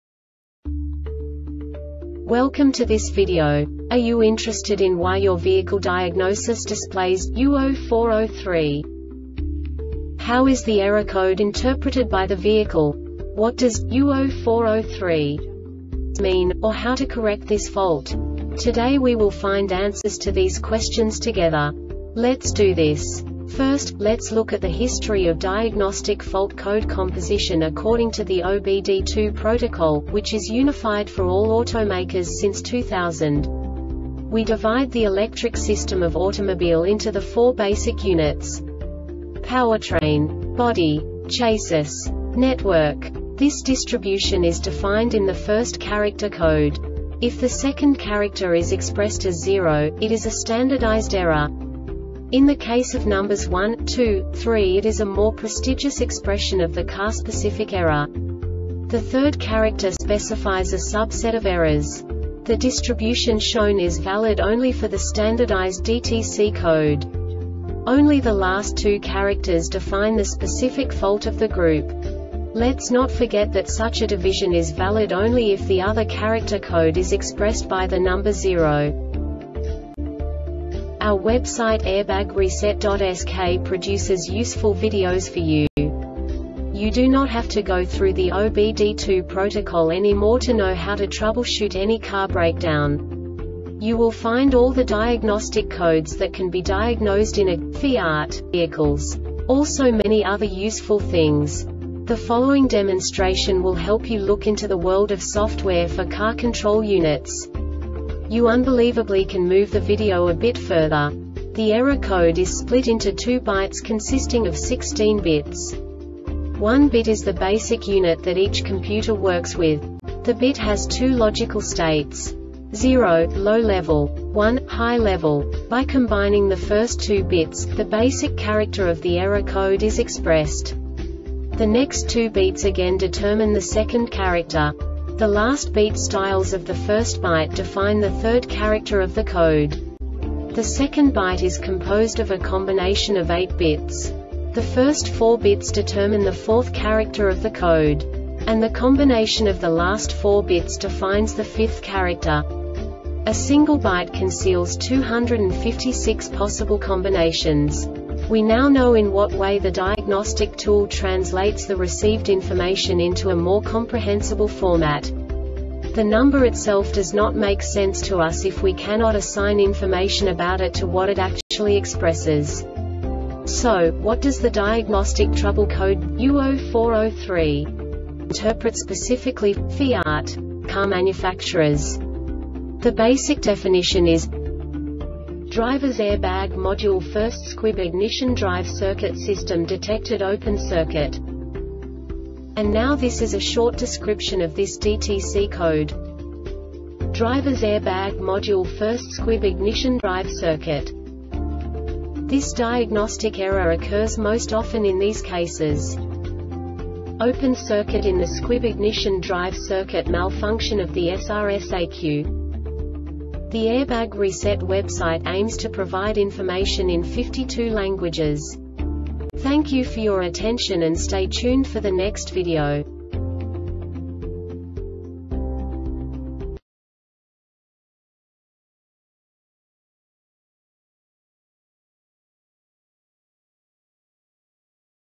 DTC Fiat U0403 Short Explanation
Contents:
0:21 Basic DTC analysis according to OBD2 protocol standard.
1:48 Insight into programming
2:58 A diagnostically understandable description of the Diagnostic Trouble Code
3:05 Basic error definition

Make:
FIAT
Code:
U0403
Definition:
Driver’s air bag module (1st squib ignition drive circuit) system detected open circuit
Description:
Driver’s air bag module (1st squib ignition drive circuit)
Cause:
Open circuit in the squib ignition drive circuitMalfunction of the SRS-ECU